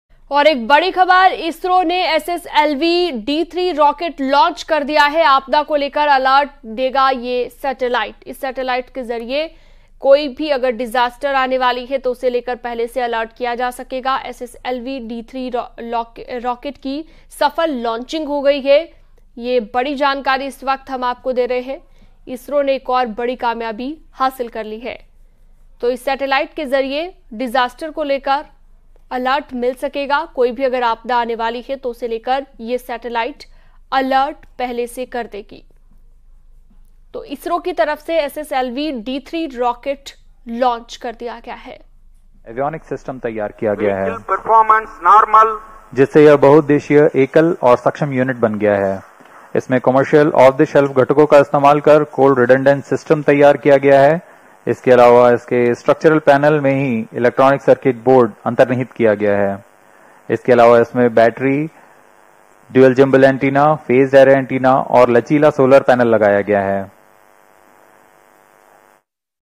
Breaking News: ISRO की बड़ी कामयाबी, लॉन्च किया SSLV-D3 रॉकेट | ISRO SSLV-D3 Launch | Trending News
ISRO (भारतीय अंतरिक्ष अनुसंधान संगठन) 16 अगस्त 2024 की सुबह 9:17 बजे श्रीहरिकोटा के सतीश धवन स्पेस सेंटर से SSLV-D3 रॉकेट लॉन्च कर दिया. इस मिशन के तहत देश का नया अर्थ ऑब्जरवेशन सैटेलाइट EOS-8 और एक छोटा सैटेलाइट SR-0 DEMOSAT को लॉन्च किया. ये दोनों सैटेलाइट्स पृथ्वी से 475 किलोमीटर की ऊंचाई पर स्थित गोलाकार कक्षा में स्थापित किए जाएंगे. इस लॉन्च को ऐतिहासिक माना जा रहा है.